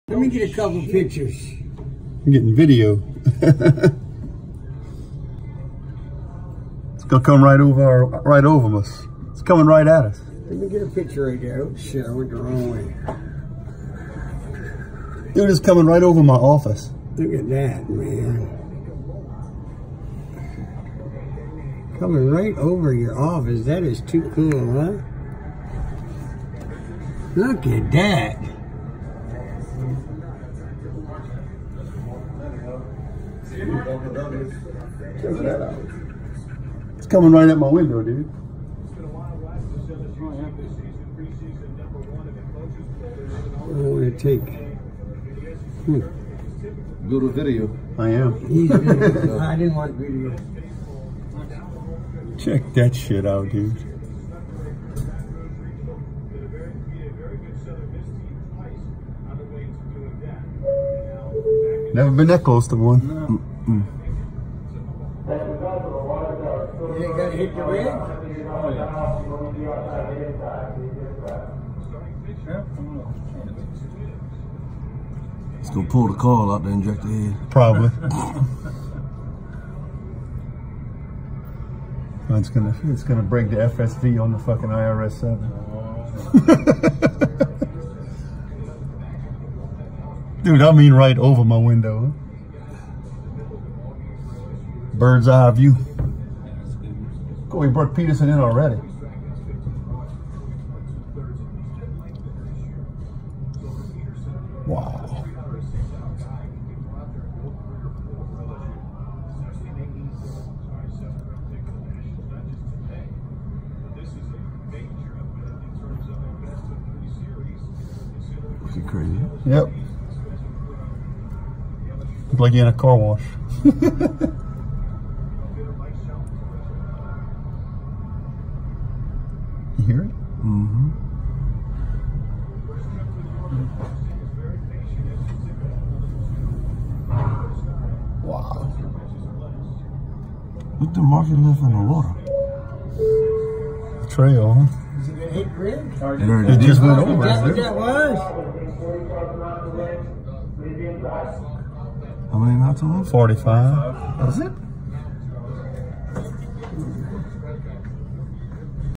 Waterspouts in the Gulf of Mexico
Video came from my dad...the next two are from the oil rig next to my dad.